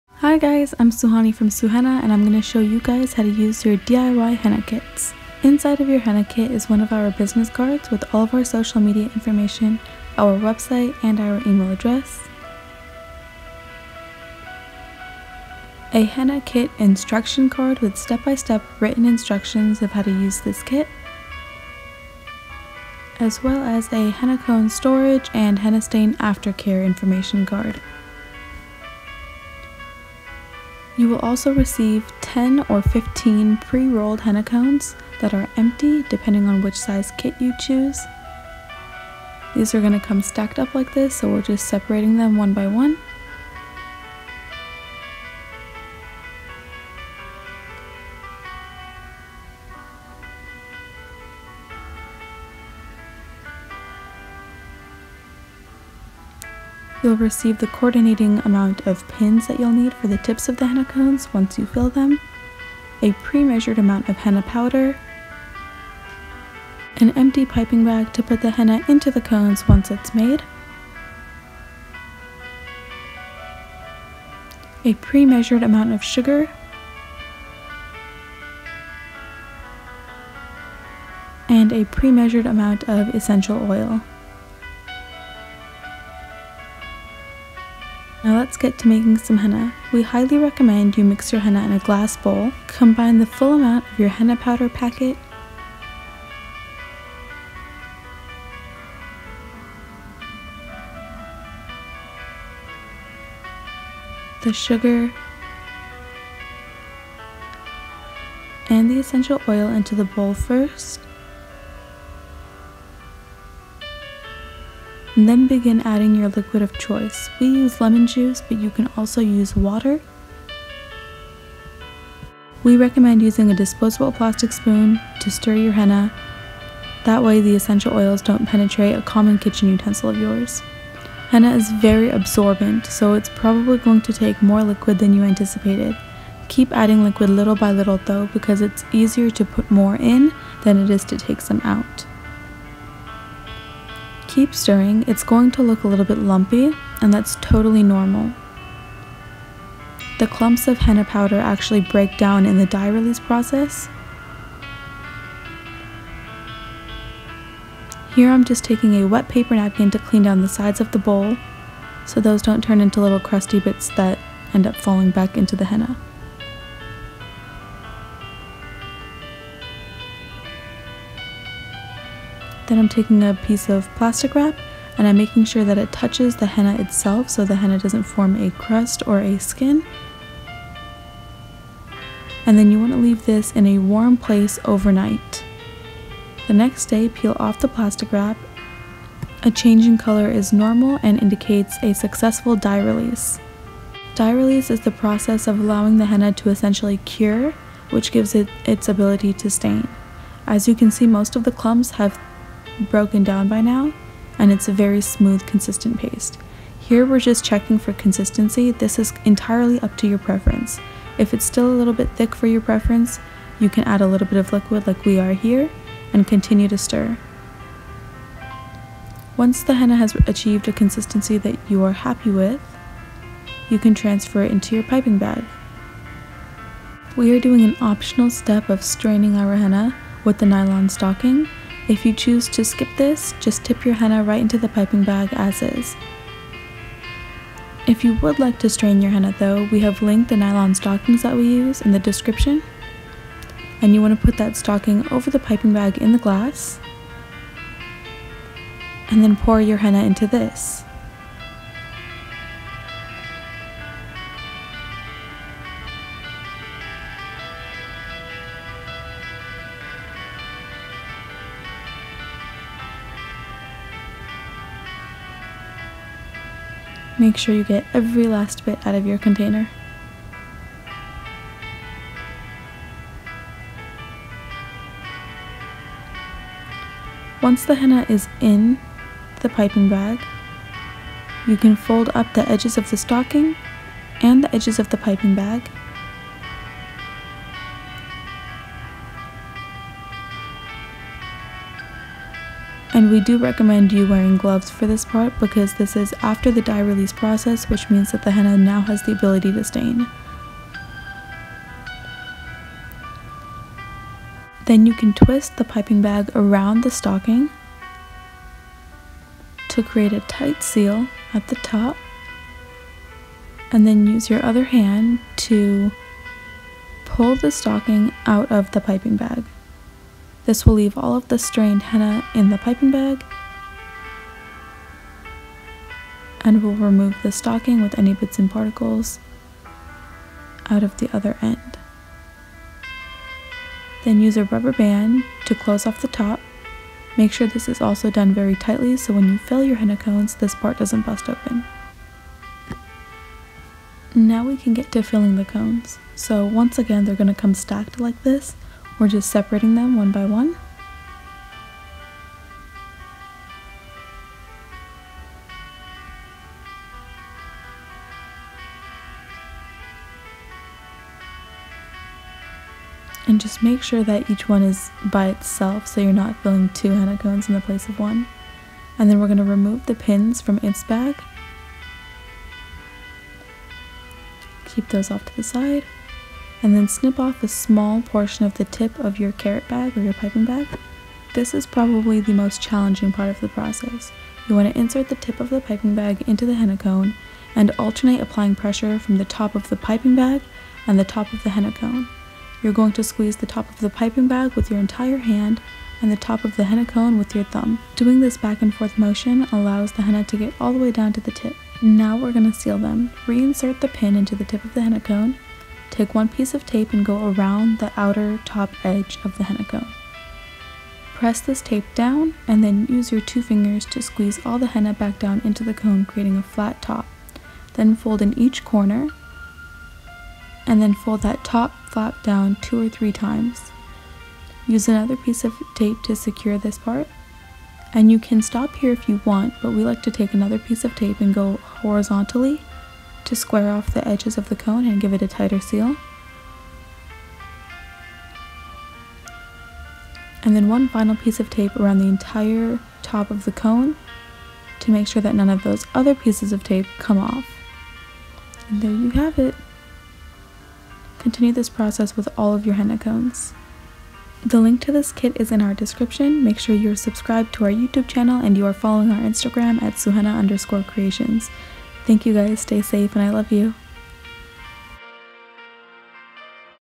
DIY HENNA KIT instructions / SUHENNA
a step by step video to learn how to make fresh henna using our henna kits!